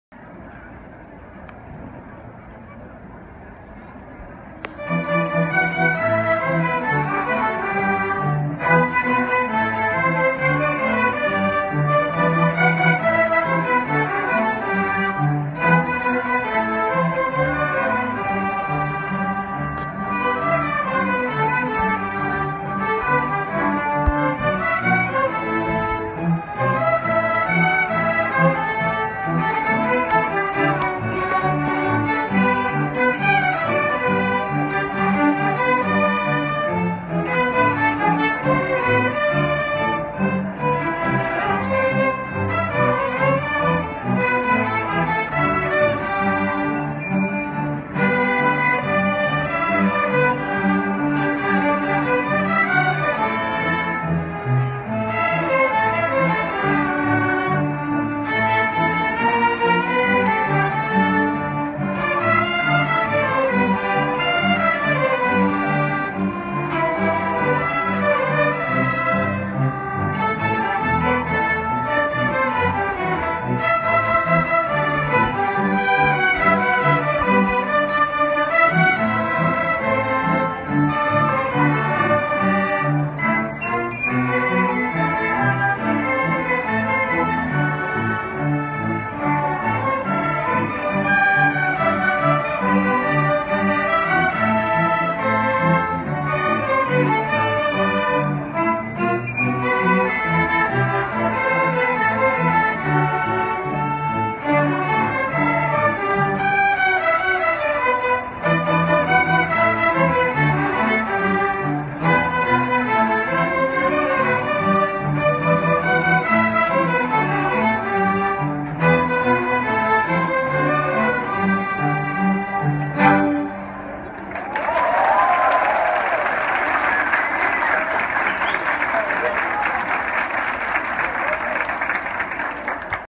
Concerto no II Seminário para Alfabetização na Idade Certa
Participação da Orquestra Maré do Amanhã em concerto no seminário promovido pela UFRJ e pela SEE/RJ no Centro de Convenções Sul América